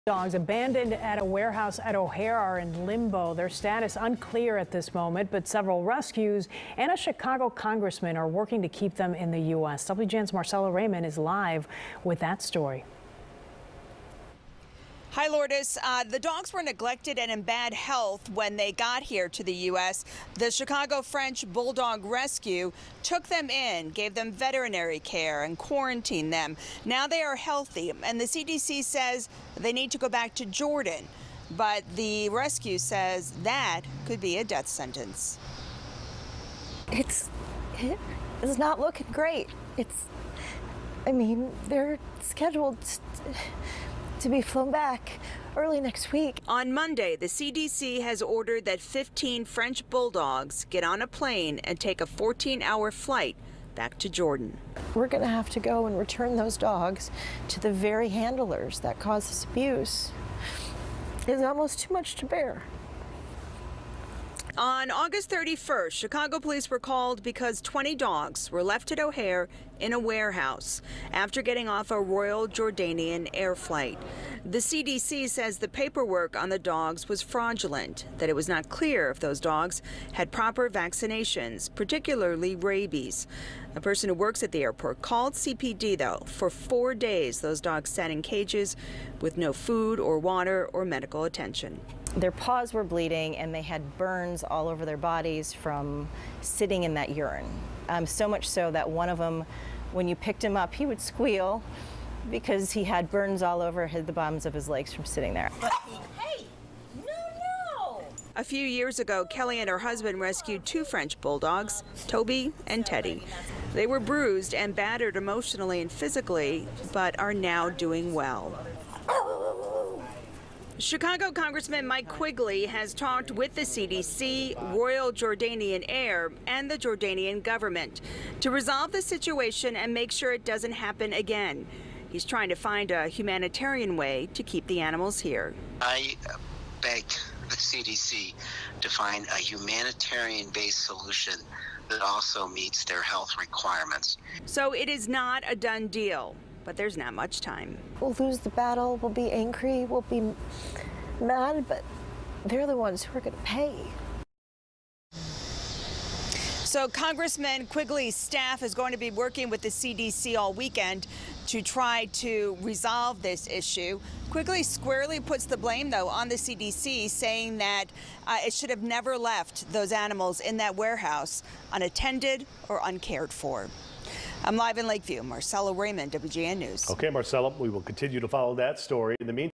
Rescue group fights to keep 15 French bulldogs in US after CDC says they have to go back to Jordan
Twenty dogs who were abandoned at a warehouse at O’Hare Airport are in limbo as a rescue group fights to keep the dogs in the United States.